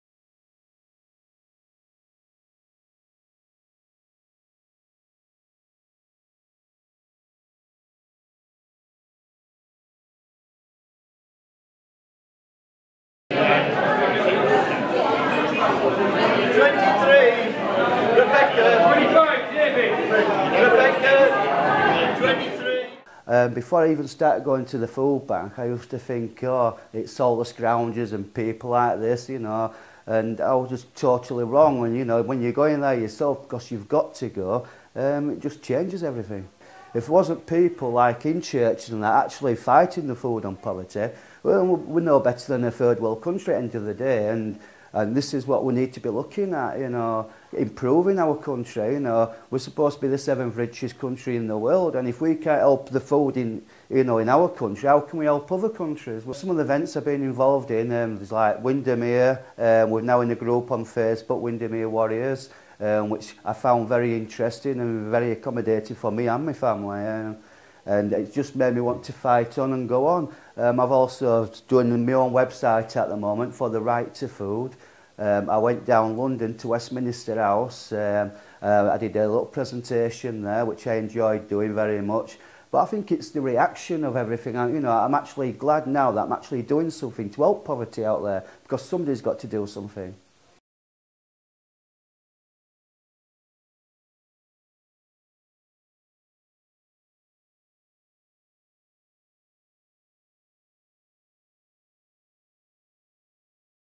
Church Action on Poverty Sunday 2016: Martin's story
Church Action on Poverty helps people in poverty to speak out , tell their stories and make a difference.
With our support, Martin - a lone parent from Halifax - has spoken out nationally about the problems in the benefits system which left him and his children dependent on a food bank.
In this video, Martin tells his story, and explains how you can make a difference by supporting Church Action on Poverty with your donations.